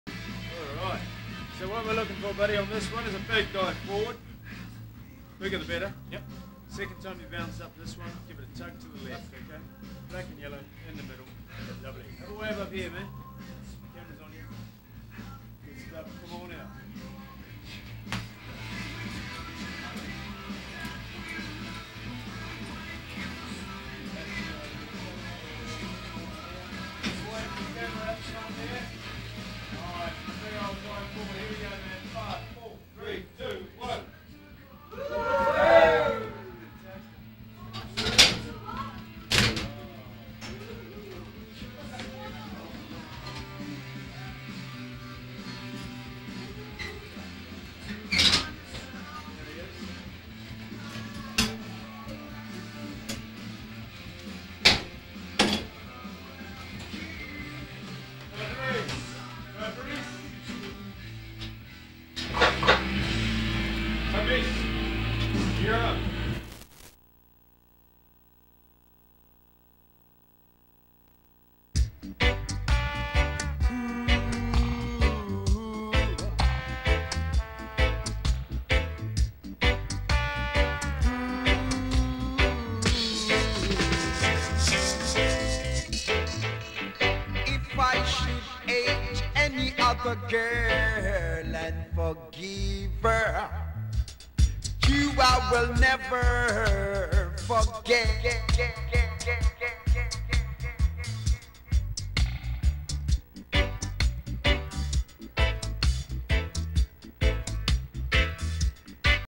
My Nevis Highwire Bungy Jump (2003)
AJ Hackett's "The Nevis" - 134M Bungy Jump, Queenstown, New Zealand